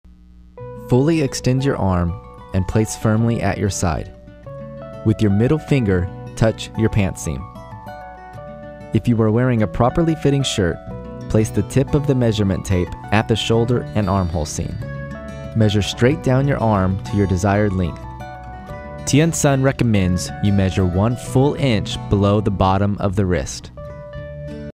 Tien Son How To's: Jacket Sleeve Length
Learn how to have a friend measure your jacket sleeve length for a custom tailored suit jacket or sports coat from Tien Son Custom Tailored Suits. For more information and available products, visit our